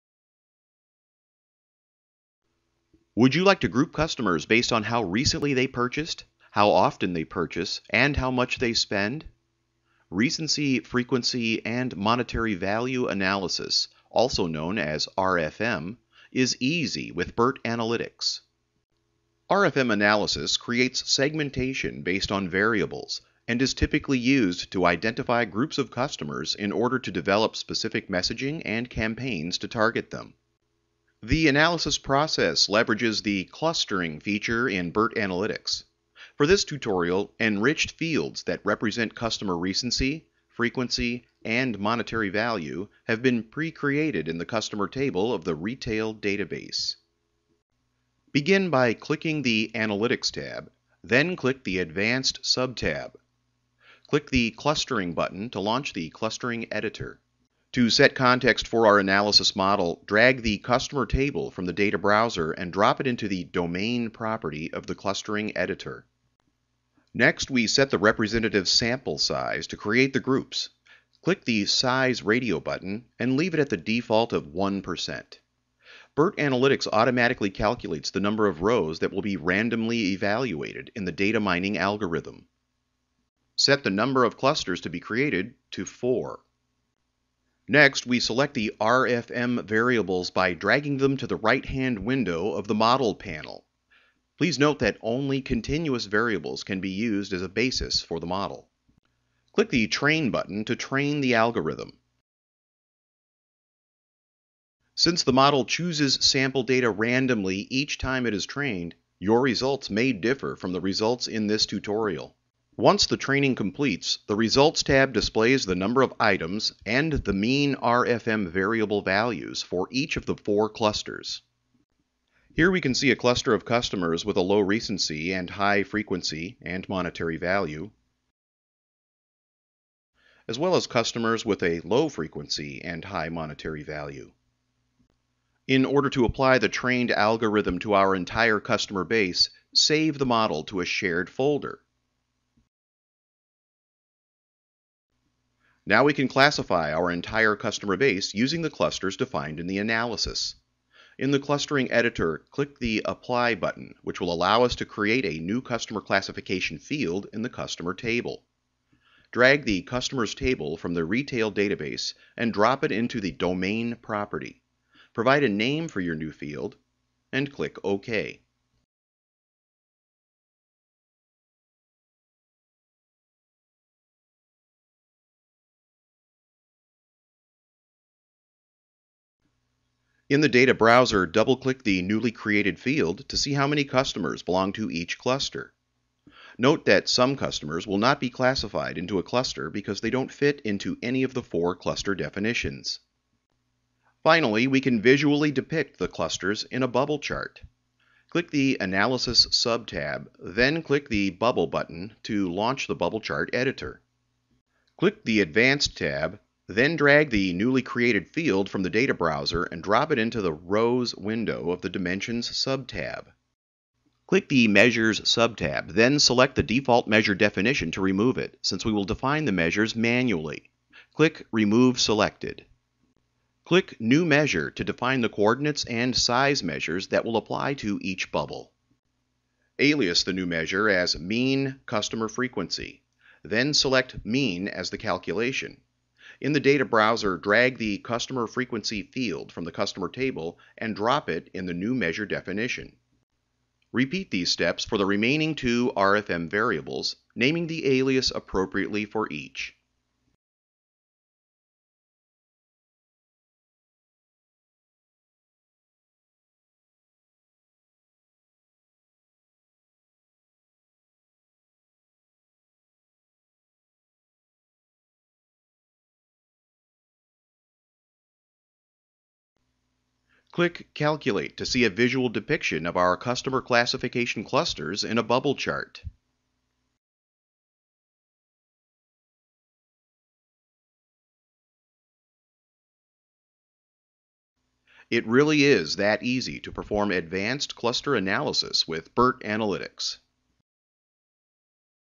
RFM Customer Value Analysis using Clustering
Would you like to group customers based on how recently they purchased, how often they purchase, and how much they spend? Recency, Frequency and Monetary Value analysis, also known as RFM, is easy with BIRT Analytics!